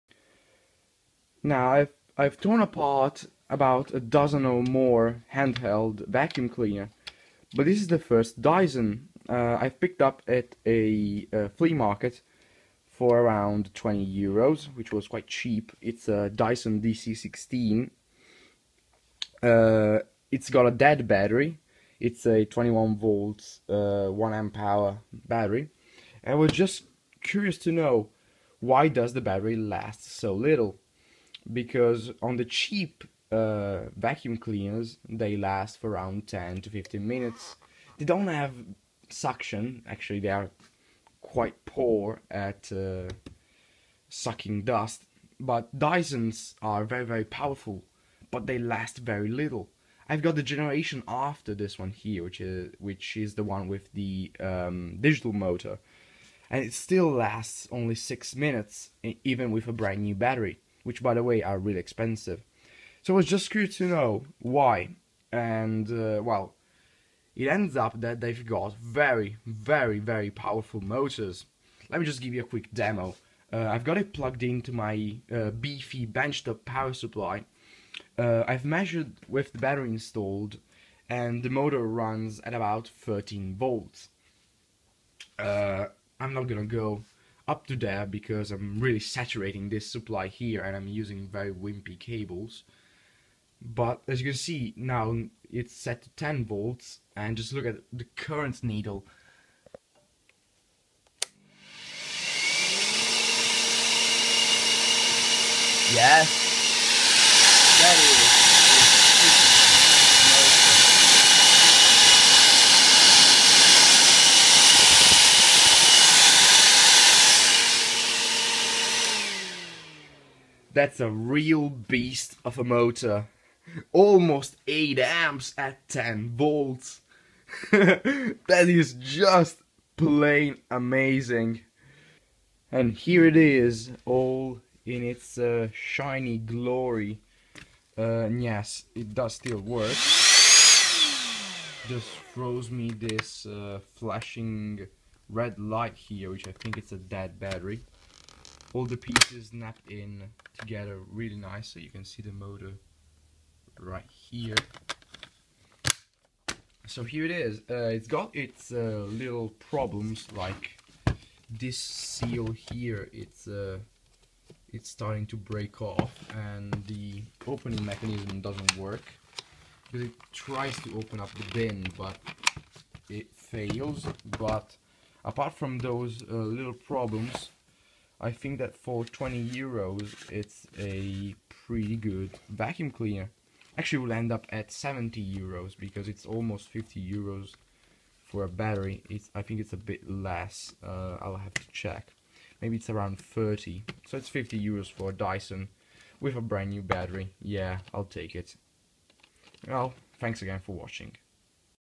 Dyson DC16 Quick Teardown and Motor Test, why the battery lasts so little?
UPDATE: Got a New battery for 35€ and now is as good as new!

I got it for 20€, it has a dead battery.
I was curious to know why the battery lasts so little, so I took it apart and discovered that it uses a very very very beefy motor!
good job Dyson!